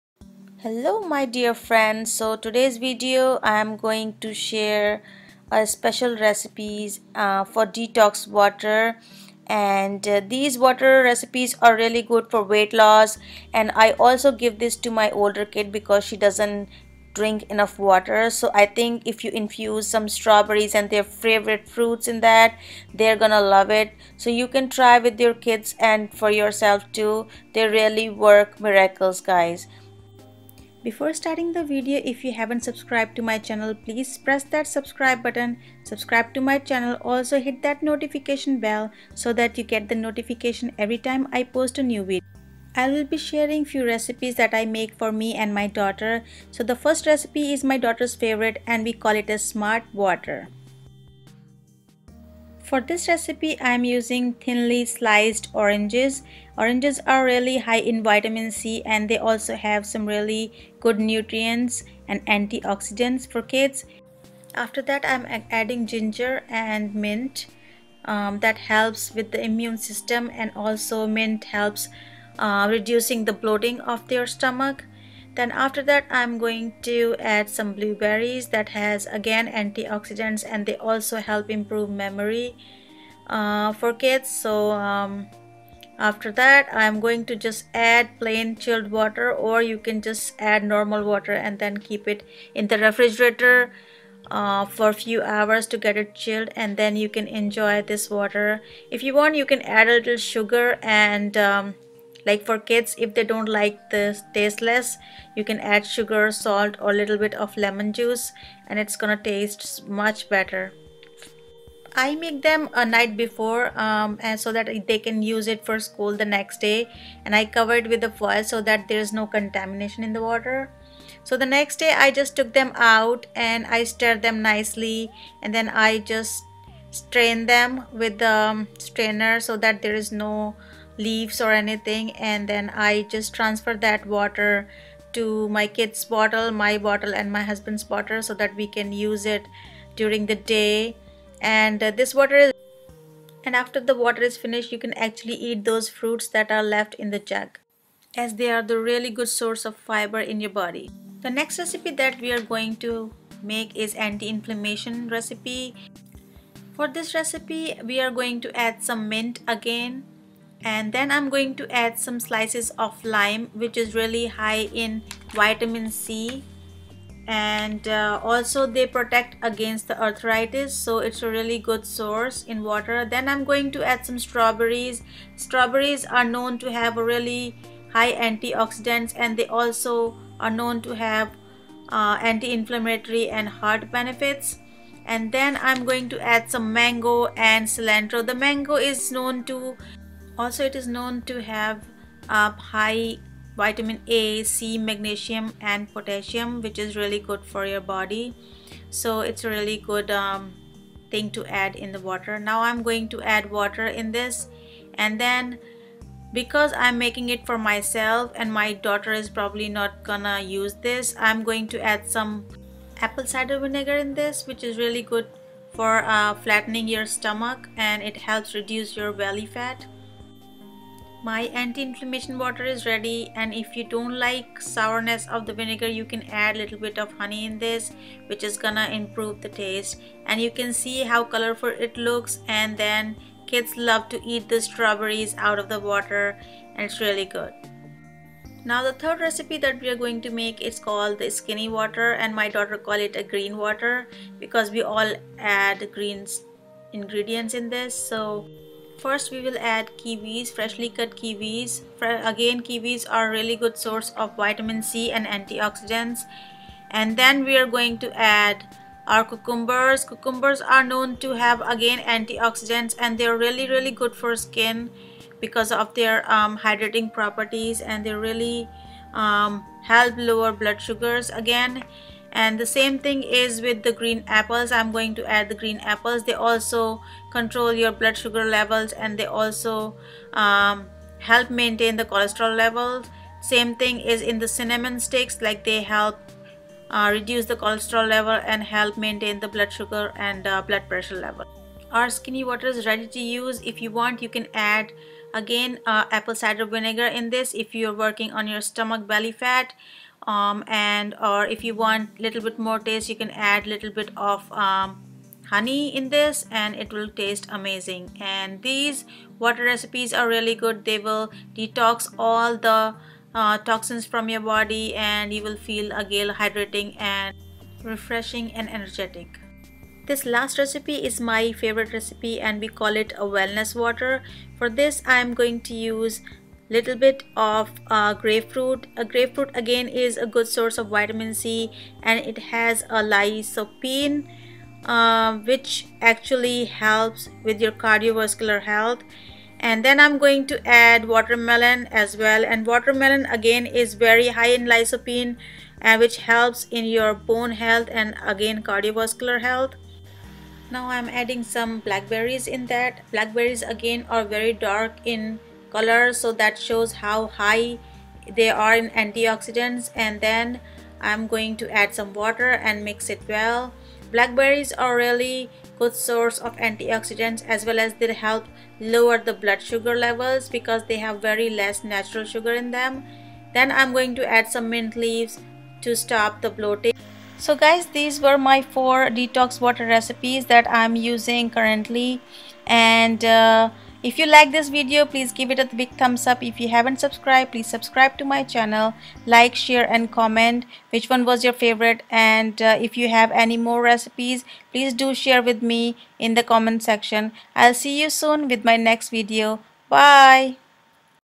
बिना किसी एक्सरसाइज के पतला होने के लिए इस वीडियो को देखे DETOX WATER for WEIGHT LOSS
बिना किसी एक्सरसाइज के पतला होने के लिए इस वीडियो को देखे WEIGHTLESS DETOX WATER RECIPES. BELLY FAT BURNERS--BODY CLEANSING--ANTI INFLAMMATORY and ANTI AGING RECIPES-- INCREASES MEMORY AND IONS in YOUR BODY AND KEEPS YOU HYDRATED ... 4 RECIPES THAT YOU WILL LOVE...